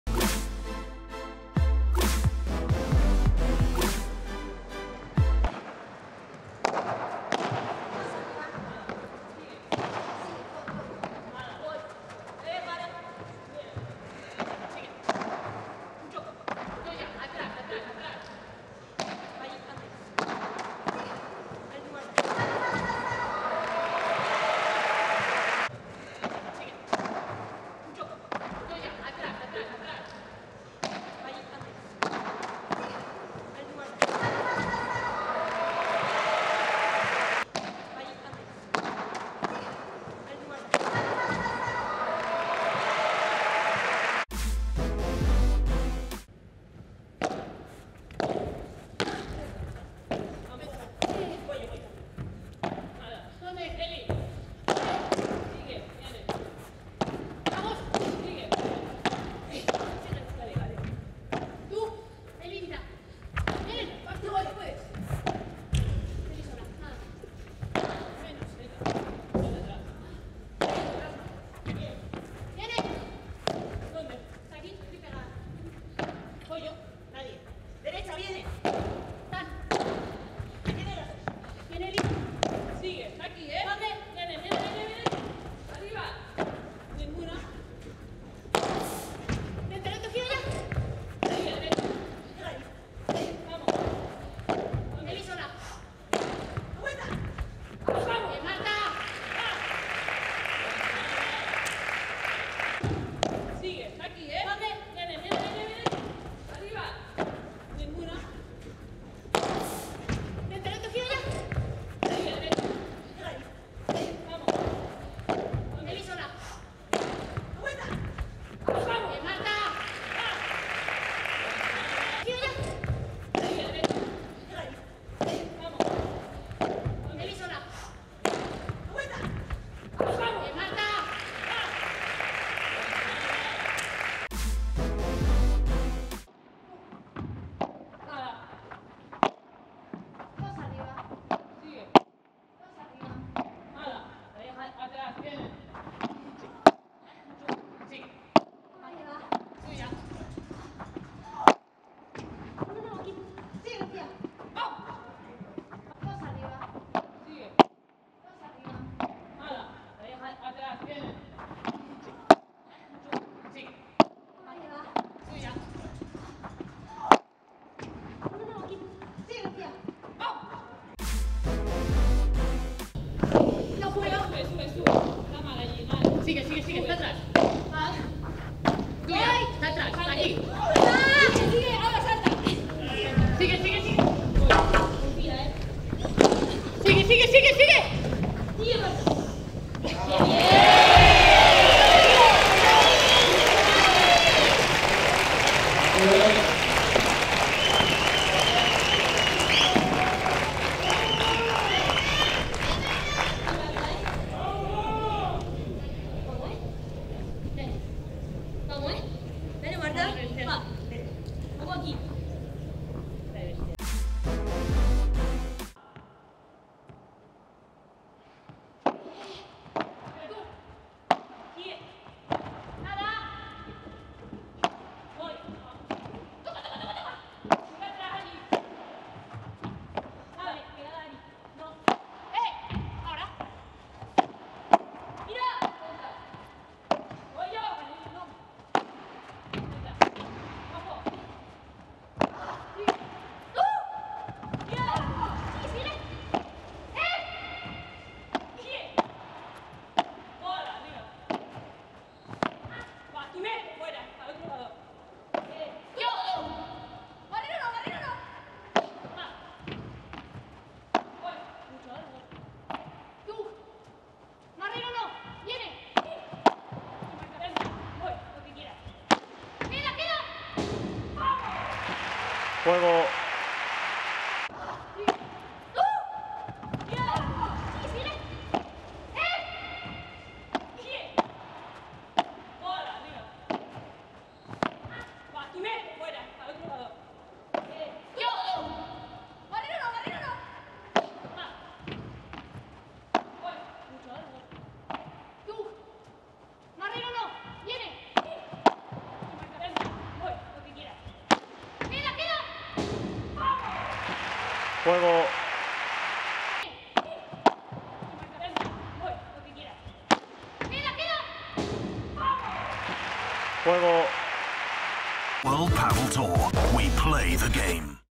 Top5 Puntazos Marta Marrero 2020 | World Padel Tour
Potencia, fuerza y explosividad son solo algunas de las características de Marta Marrero. Es una de las jugadoras más temidas dentro del 20x10, y más respetadas fuera de la pista. Toda una número 1 que en 2020 comenzó luchando de la mano de Paula Josemaría, y terminó reencontrándose con Marta Ortega en busca del trono mundial. En su búsqueda, dejó para el recuerdo puntazos como estos cinco.